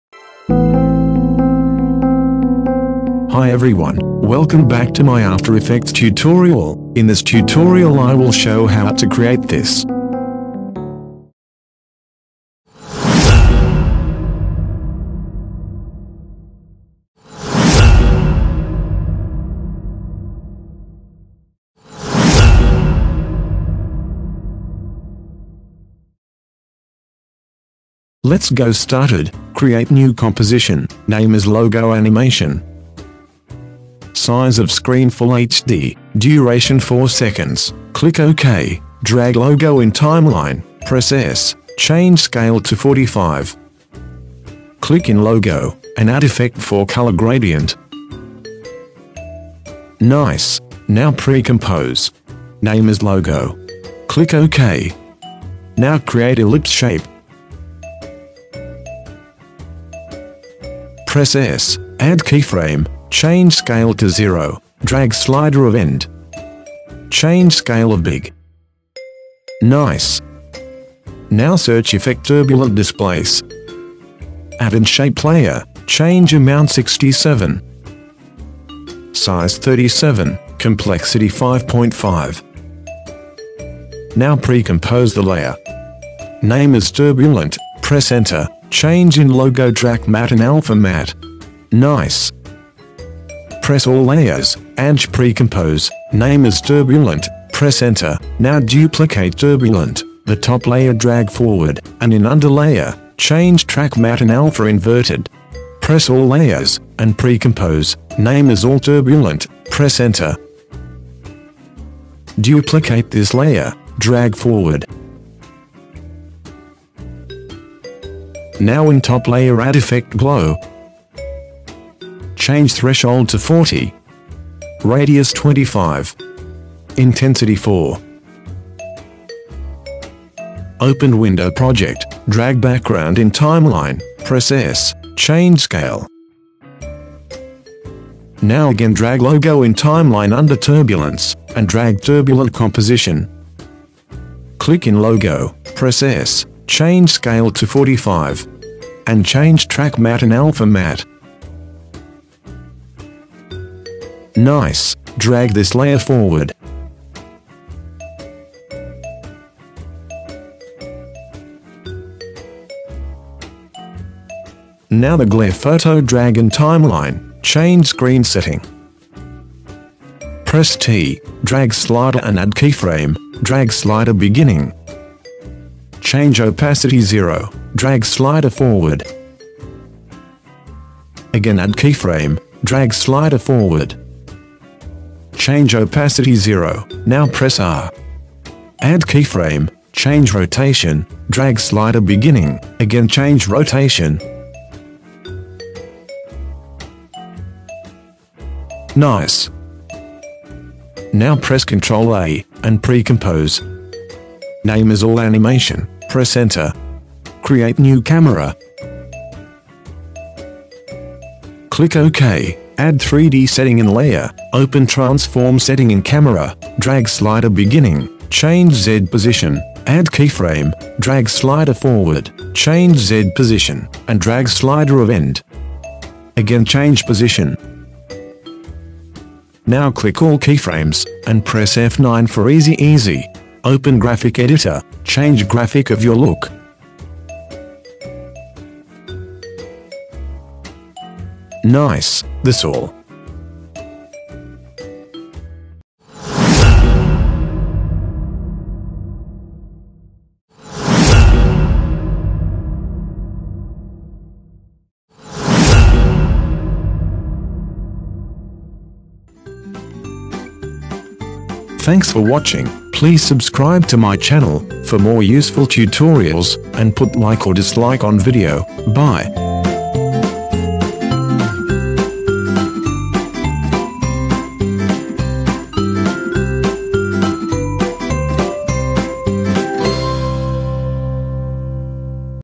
Liquid Logo Animation in After Effects - After Effects Tutorial - Easy Method
Hi everyone,welcome back to my after effects tutorial.
Today we create liquid logo animation in adobe after effects,for this need AFTER EFFECTS program,Learn how to make smooth logo reveal in AE,new easy method.

If you have any questions write in comments